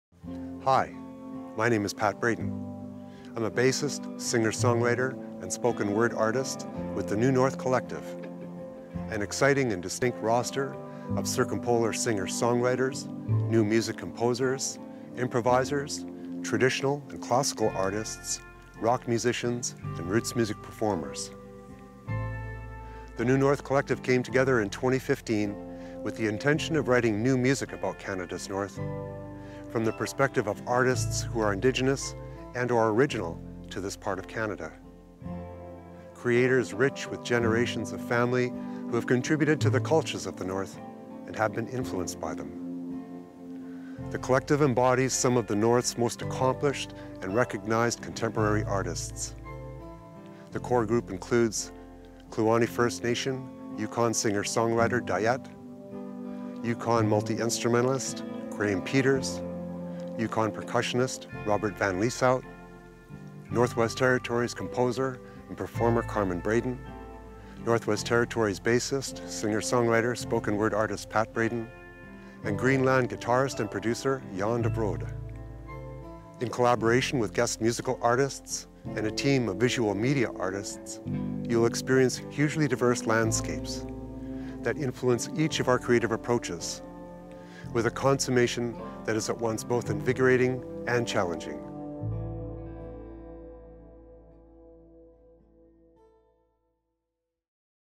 Trailer: A Circumpolar Digital Concert Series with New North Collective
Music From the Edges of Canada is very proud to present this trilogy of digital concerts by New North Collective.

Tune in right here at 7:30PM Pacific on Thursdays
November 24, December 1 and December 8.

New North Collective is an exciting and distinct roster of circumpolar singer-songwriters, new music composers, improvisers, spoken word artists, traditional and classical artists, rock musicians and roots music performers.

The collective includes: Diyet - Kluane First Nation Singer-Songwriter / Graeme Peters - Yukon Multi-Instrumentalist / Robert Van Lieshout - Yukon Percussionist / Carmen Braden - Northwest Territories Composer, Performer / Pat Braden - Northwest Territories Bassist, Singer-Songwriter, Spoken Word / Jan de Vroede - Greenland Multi-Instrumentalist, Producer

Formed in 2015, New North Collective explore their ideas of the north - from the land to the people; from the traditional to the contemporary. Their shared music takes an in-depth look at being a northern person and artist with the impact of geography and community on the music they create collaboratively. Their music is diverse and skillful, representative of a multitude of genres. Specifically designed to discard stereotypes and push boundaries, this multi-media experience is simultaneously crowd -pleasing and thought-provoking for a diverse audience.

New North Collective is all about collaboration and inclusion. They invite northern artists to join them as guests wherever they play.